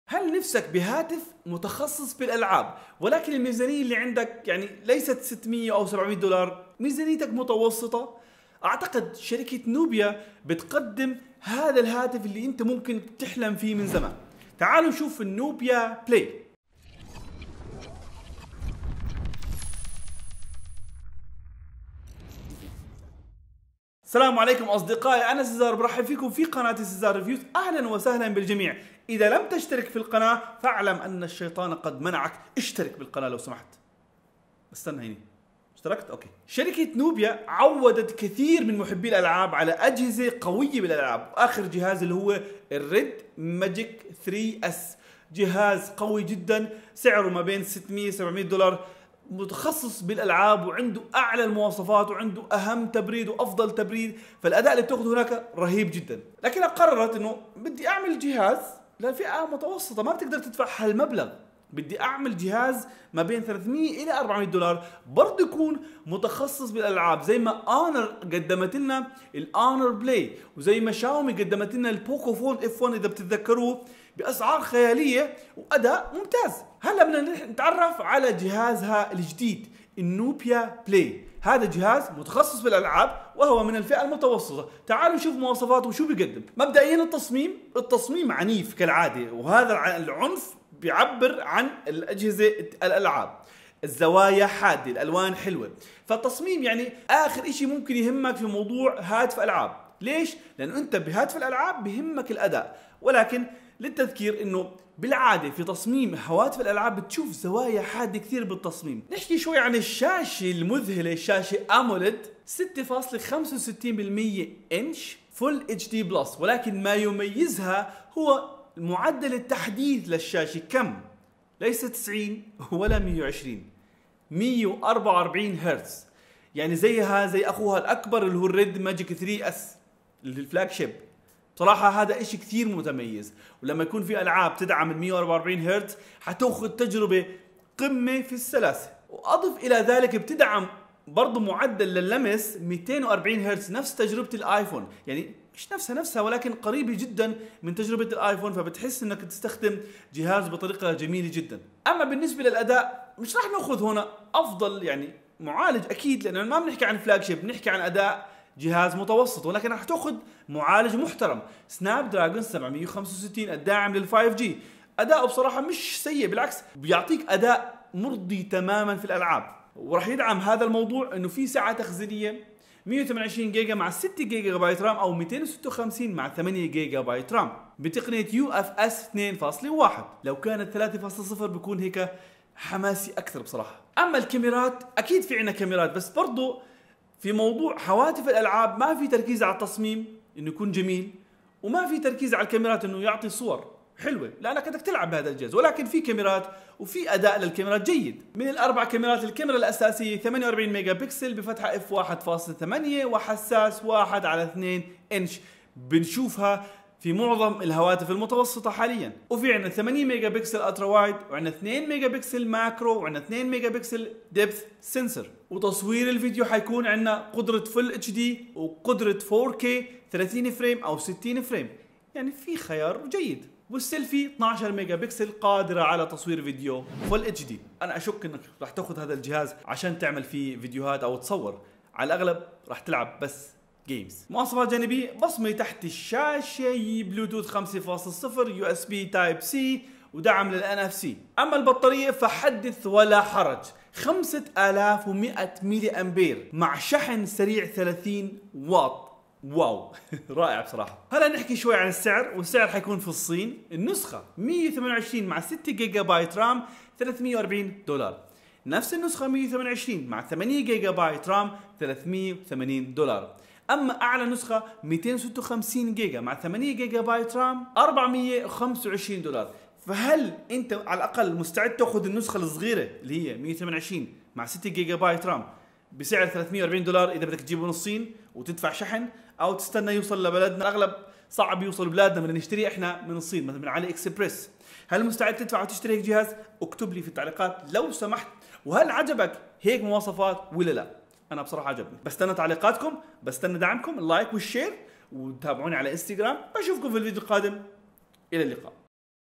ثاني هاتف متوسط متخصص للألعاب - Nubia Play
تبحث عن هاتف مثالي للألعاب
شركة نوبيا تقدم خيار صعب مقاومته
تعالوا نتعرف على أفضل هاتف ألعاب من الفئة المتوسطة
هاتف nubia play